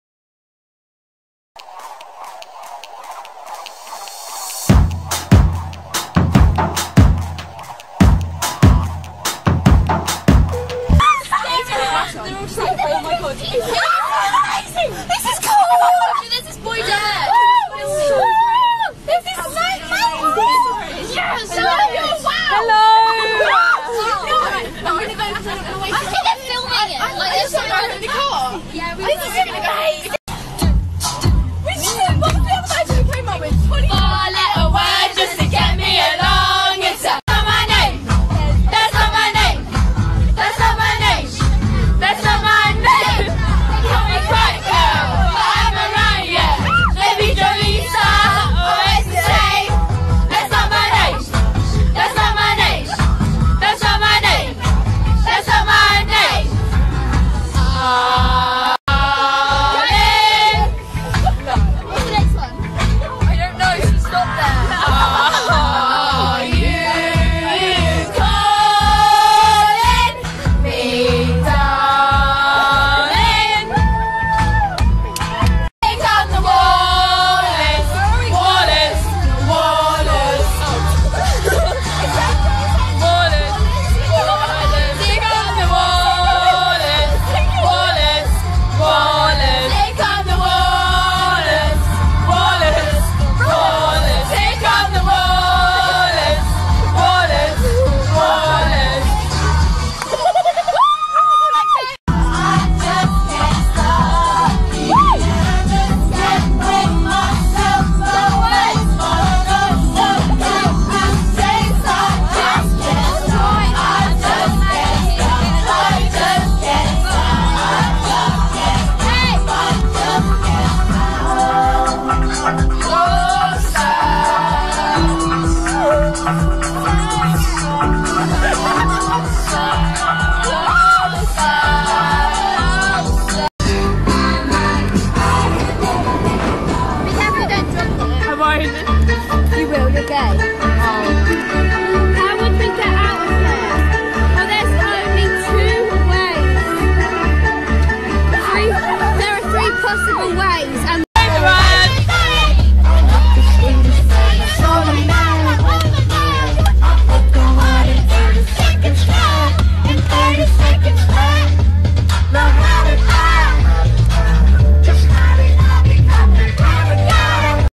Wallers Go In A Limo :O
We did this at Charlottes Party when we went from the restaurant to her house :P Good times
One step away from fame!
Uploaded By Ely x